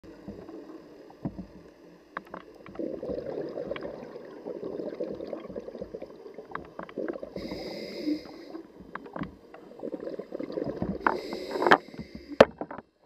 Contour Roam Lobster
Lobster off of Grenada. Sorry for the foggy lens...that's why you don't open the underwater case between dives!